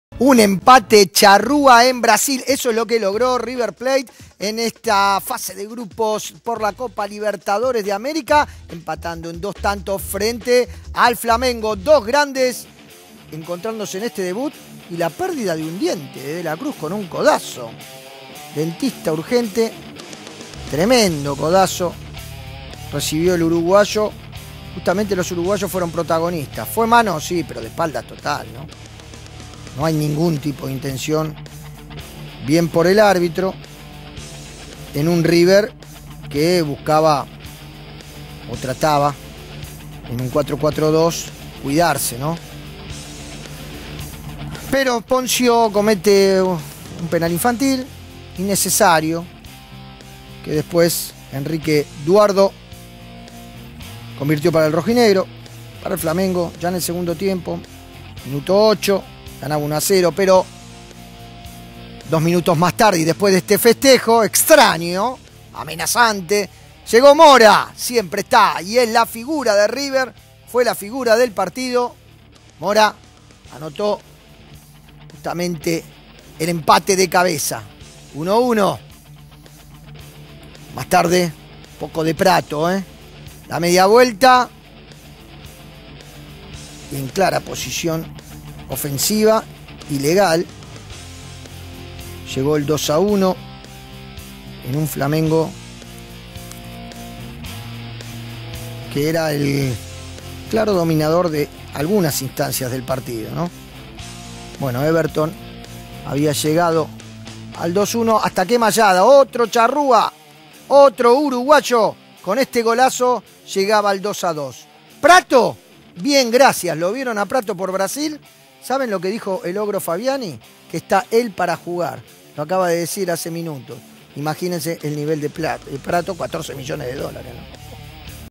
River se trajo un empate de Brasil
Emitido por TV Pública Argentina, el jueves 1 de marzo de 2018. y